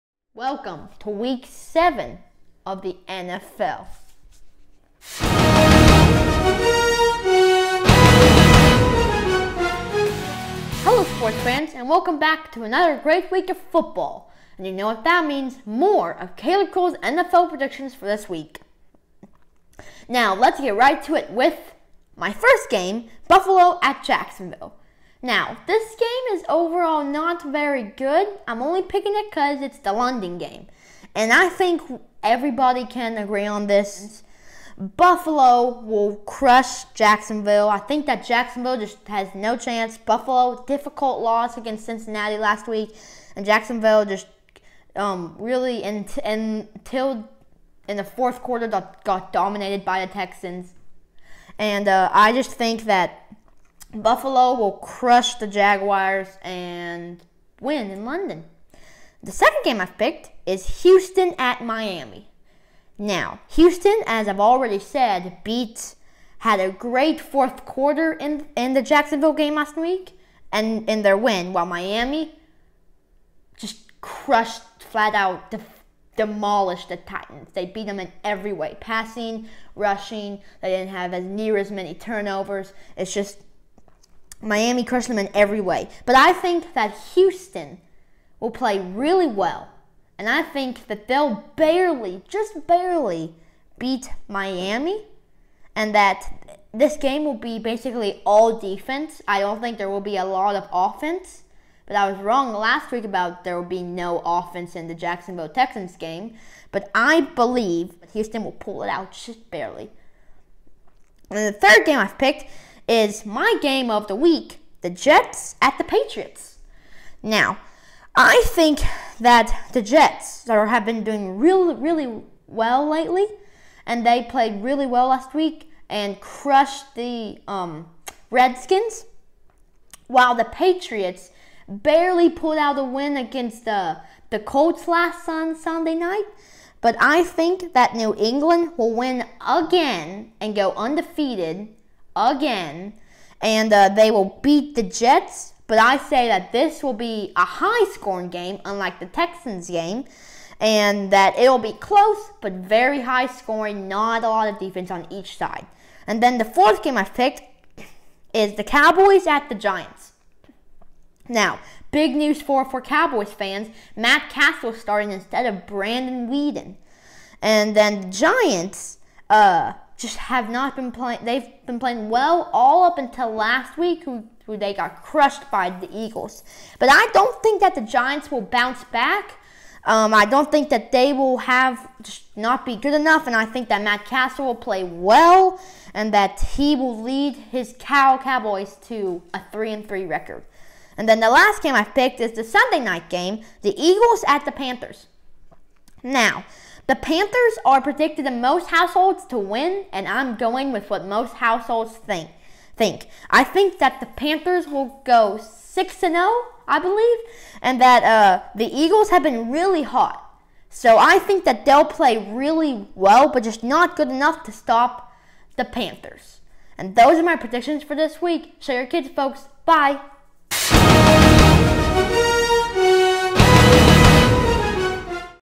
Caleb's 2015 Week 7 NFL Predictions
Texans, Cowboys, an Game and London and the Game of the week in the AFC East